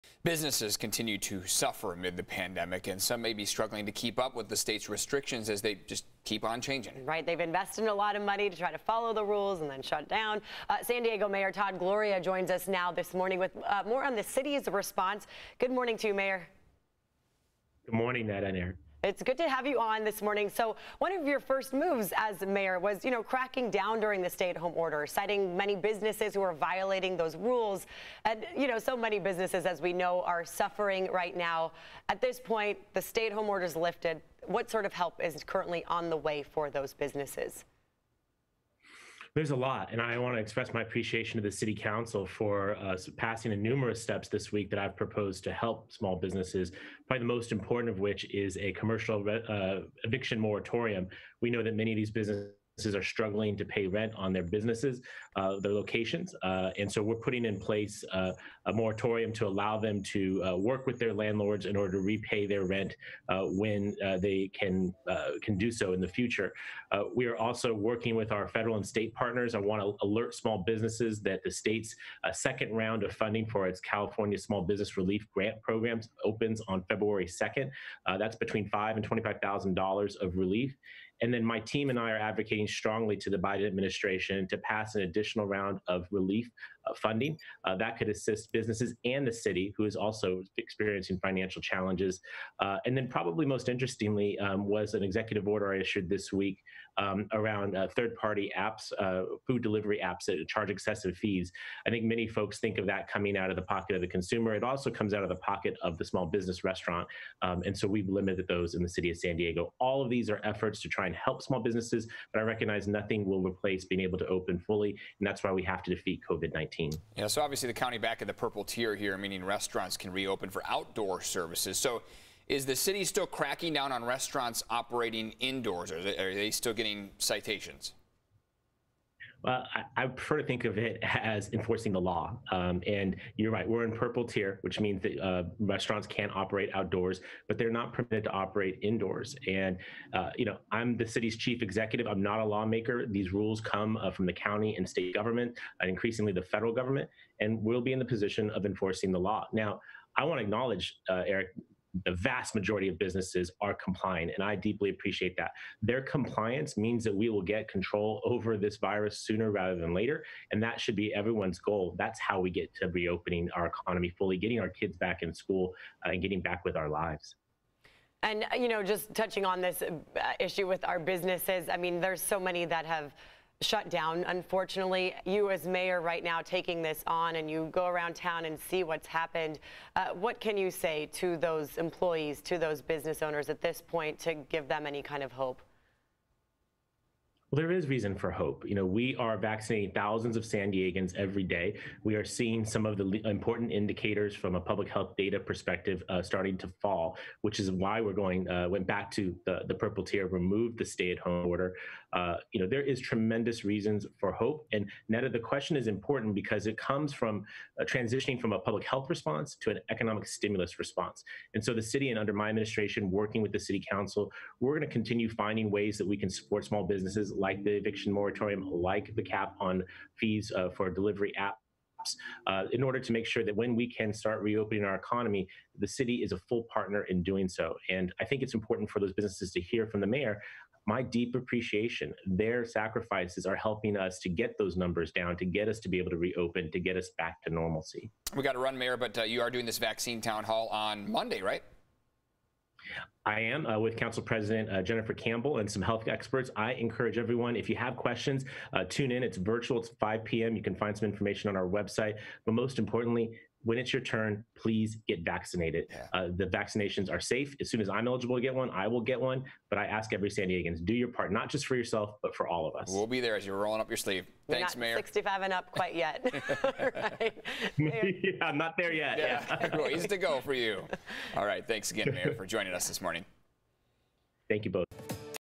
Checking in with San Diego Mayor Todd Gloria
Mayor Gloria joined the show to talk about everything from the city providing help for small businesses to a vaccine town hall.